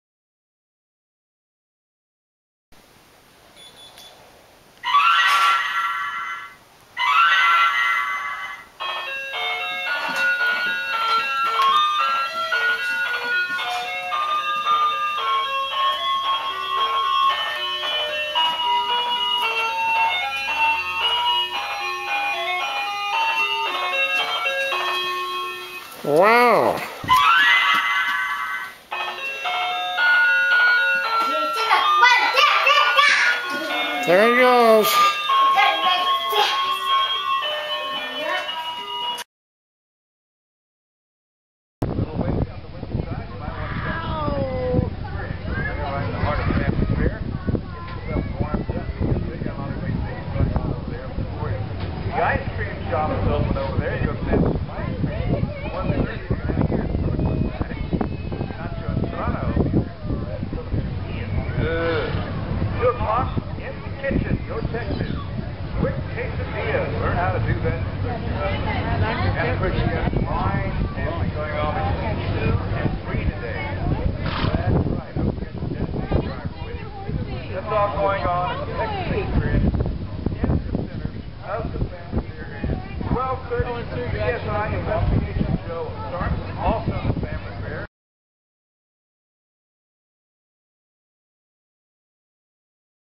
Isabel Loves Ponies 2 09
Isabel riding ponies at home and at the stock show.